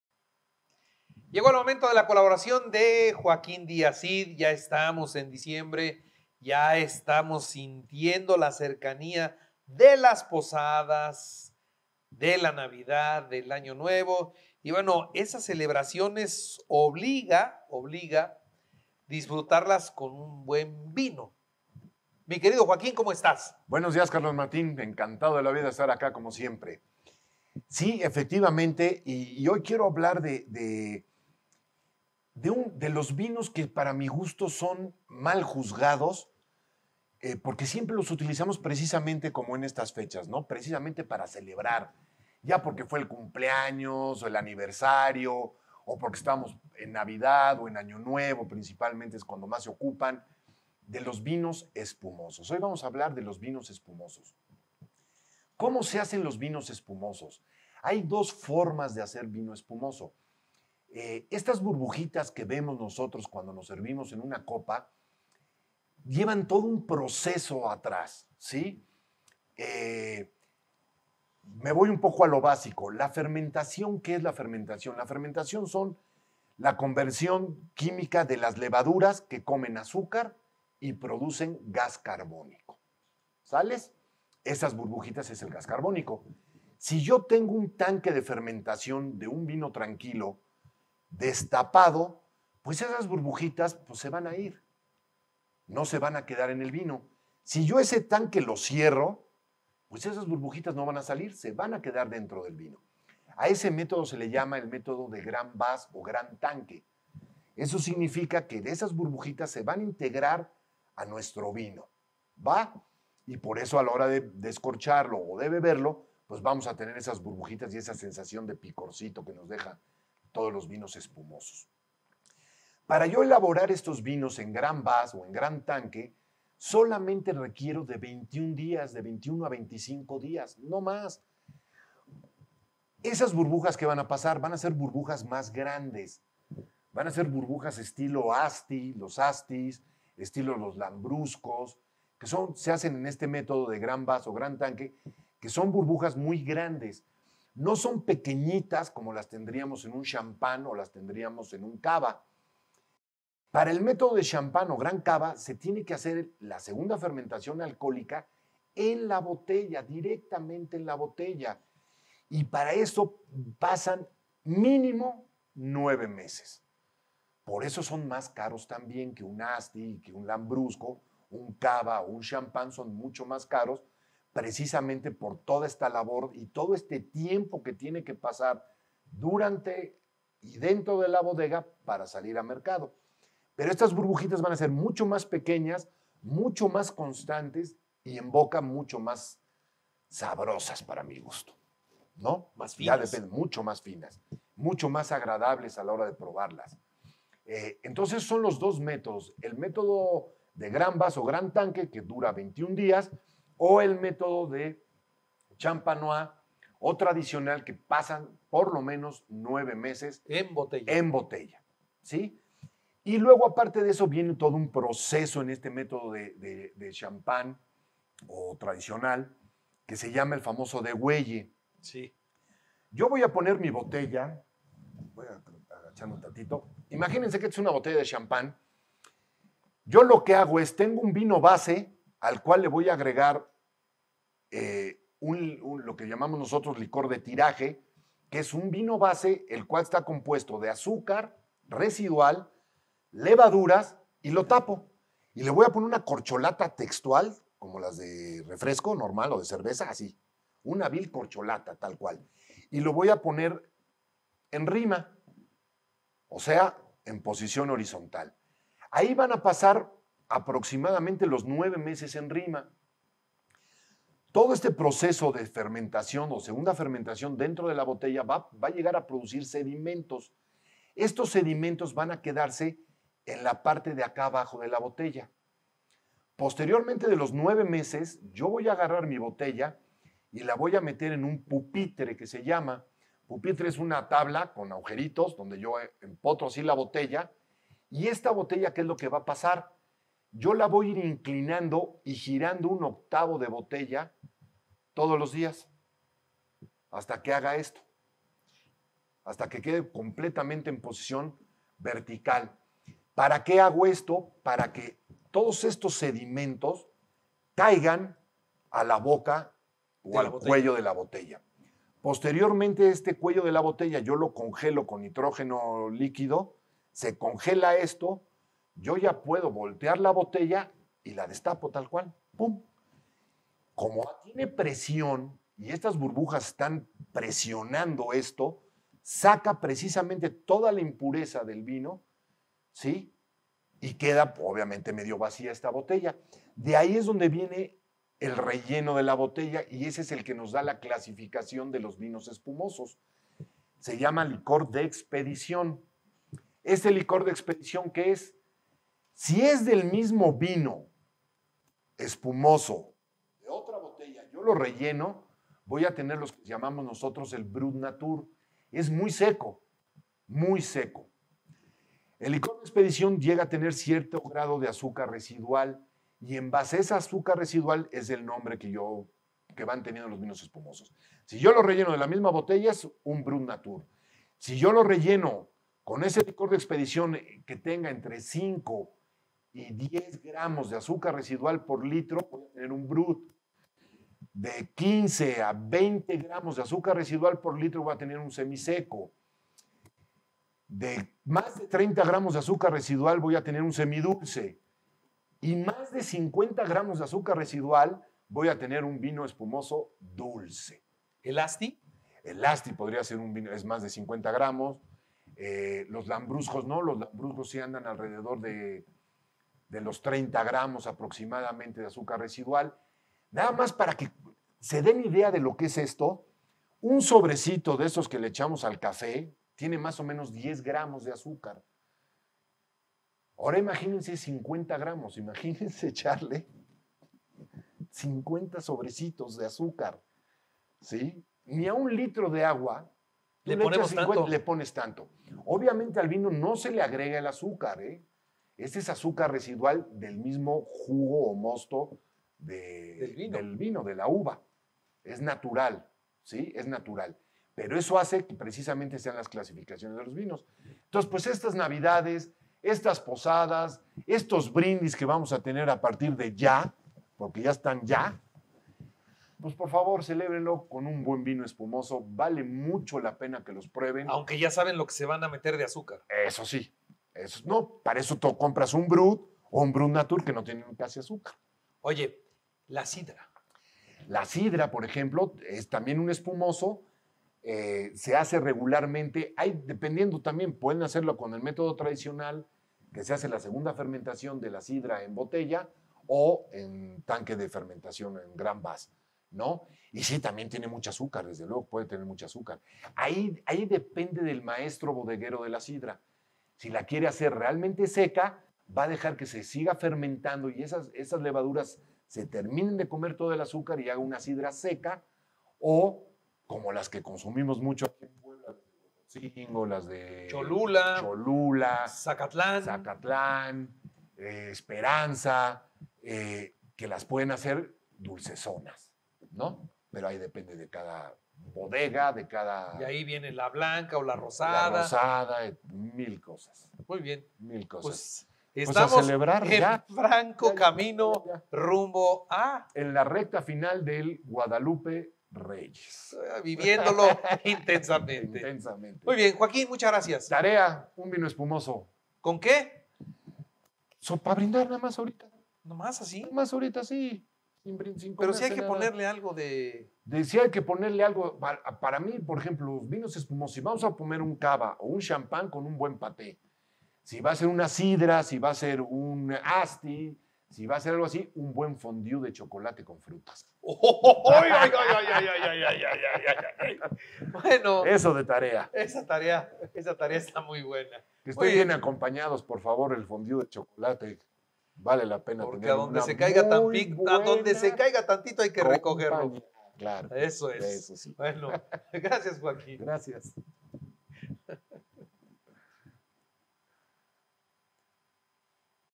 APRENDE a DISFRUTAR los VINOS ESPUMOSOS con Joaquín Díaz Cid
Joaquín Díaz Cid nos comparte TODO sobre los VINOS ESPUMOSOS. Conoce su PREPARACIÓN y los DISTINTOS tipos que EXISTEN en el mercado.